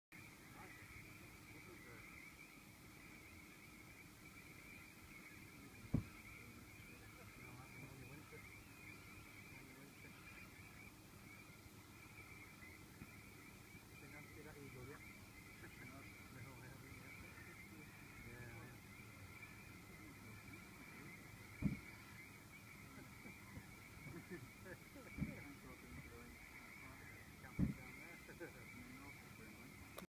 White-winged Black Tern preening at Cemlyn
iPhone scoped video of WWBT in heat haze at Cemlyn 17th June 2014